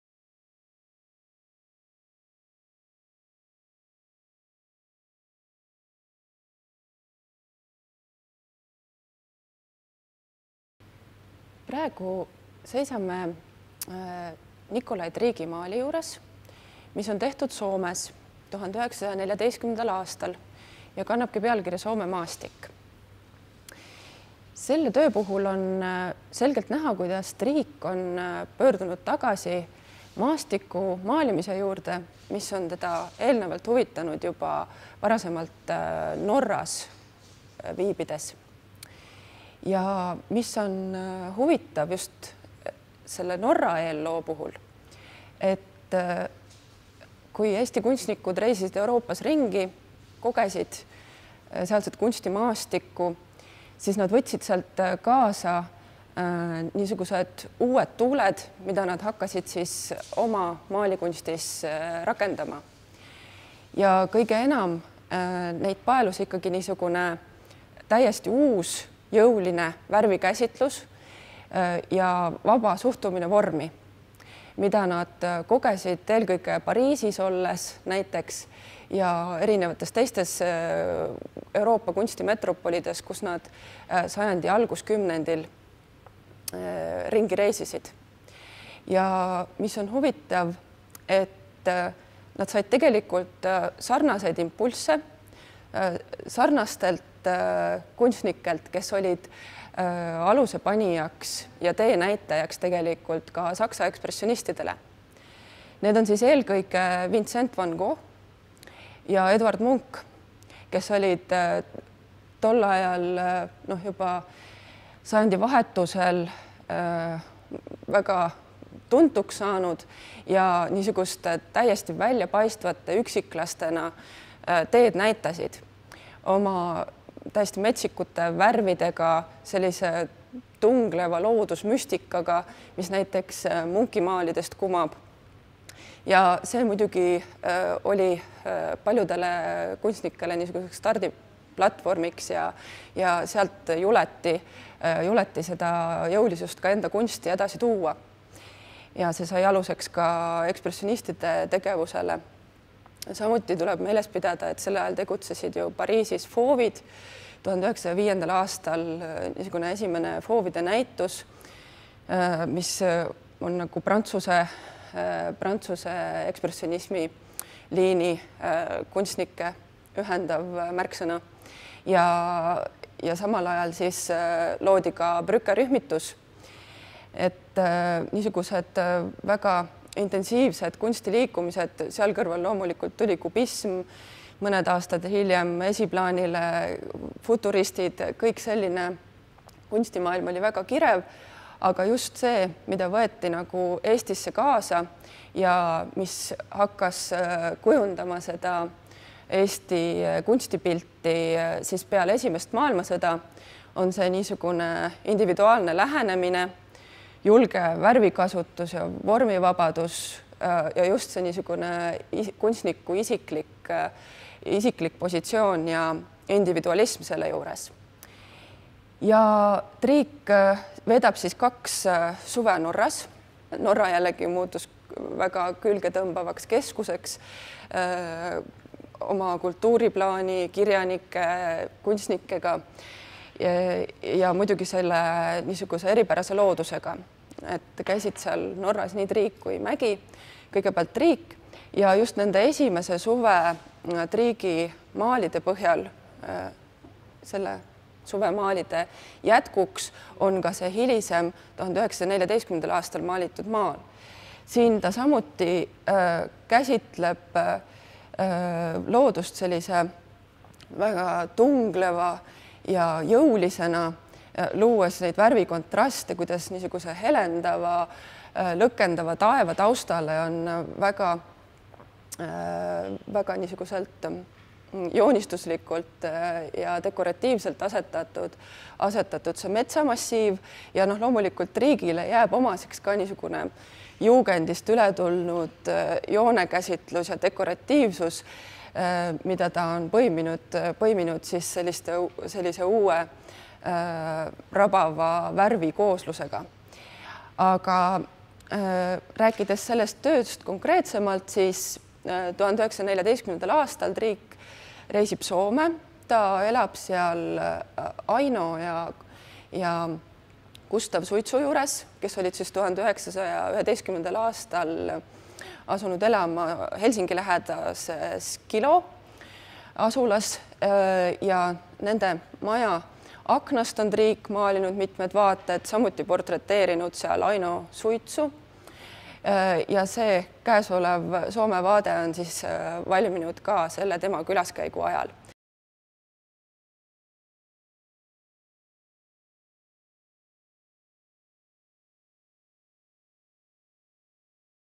Liis Pählapuu - Nikolai Triik "Soome maastik"
Näituse "Saksamaa metsikud" kuraator Liis Pählapuu räägib Nikolai Triigi 1914. aastal valminud maalist "Soome maastik".